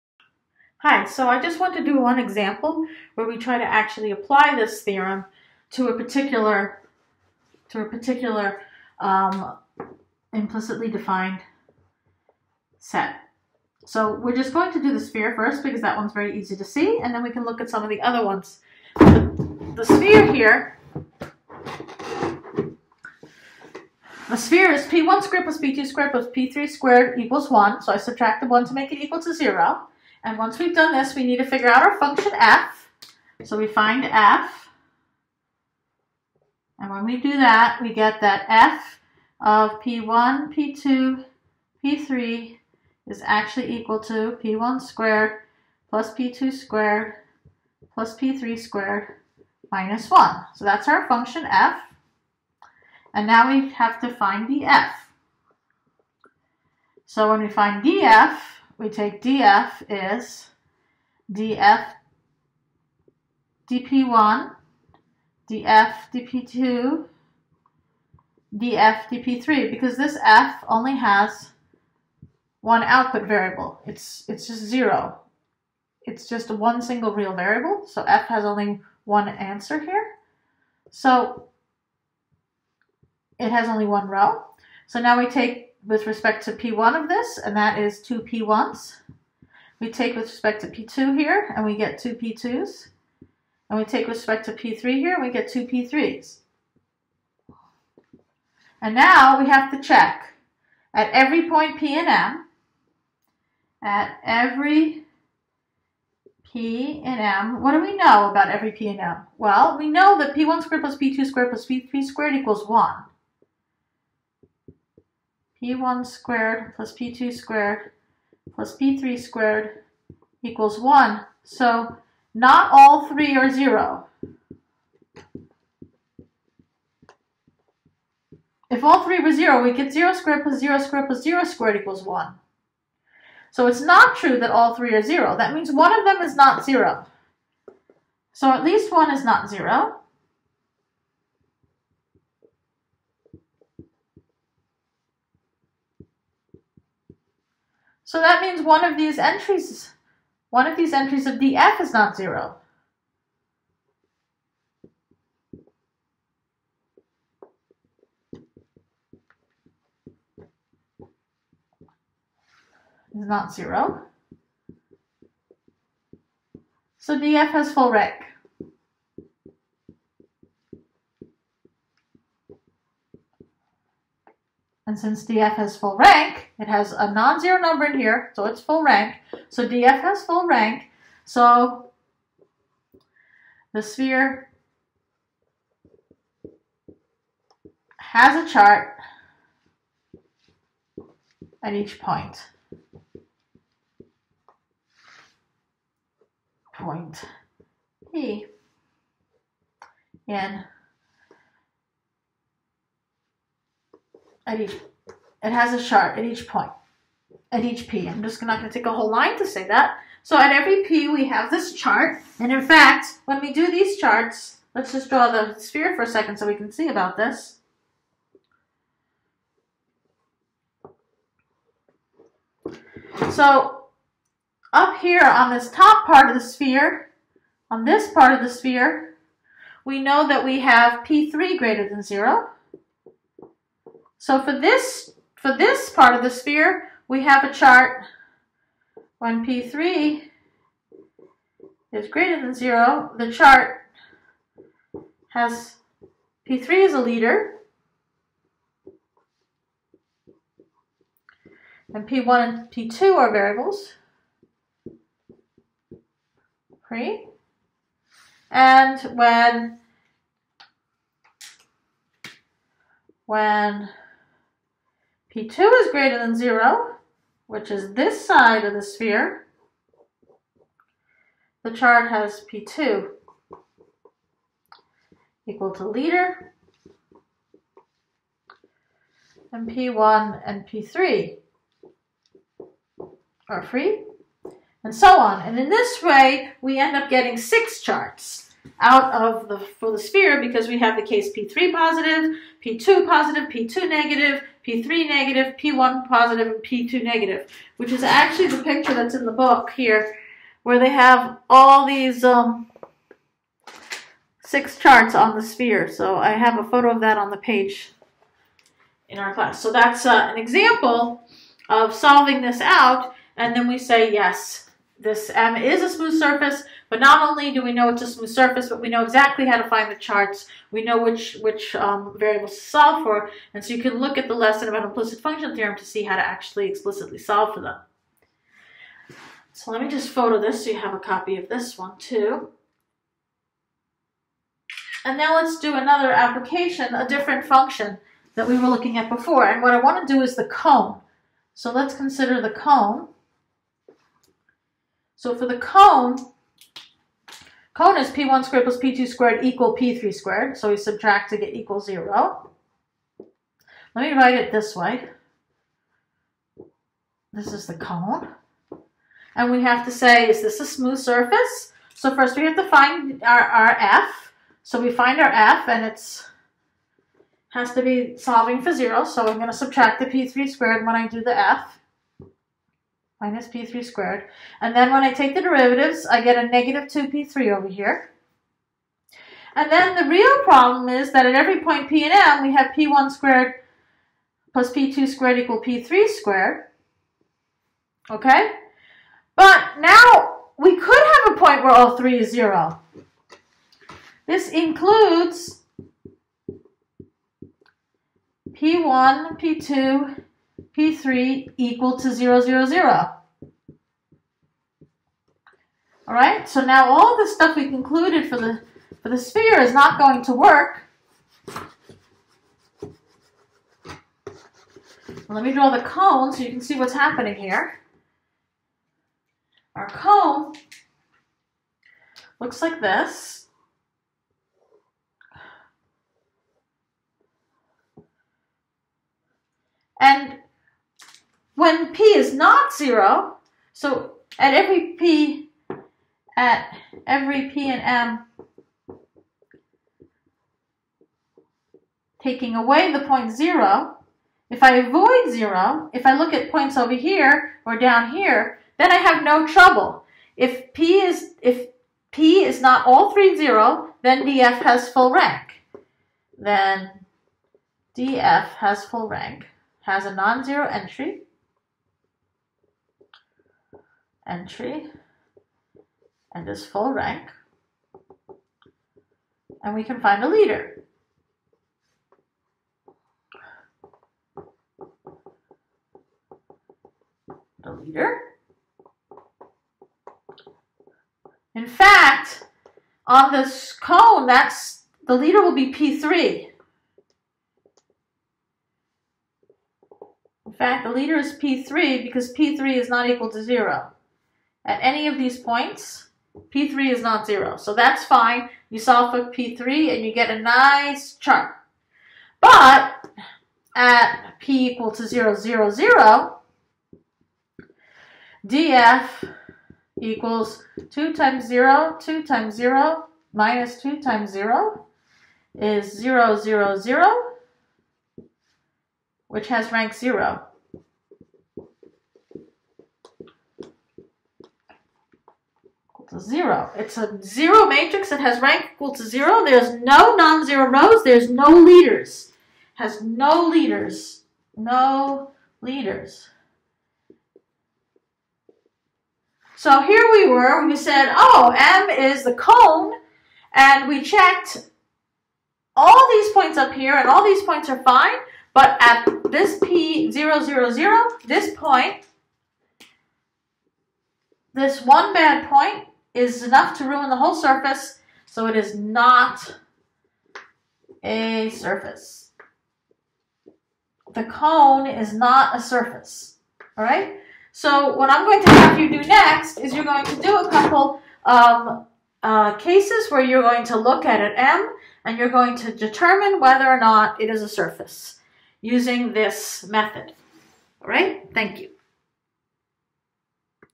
SurfacePart5
Using the Implicit Function Theorem to check if two sets are smooth surfaces: the sphere and the cone.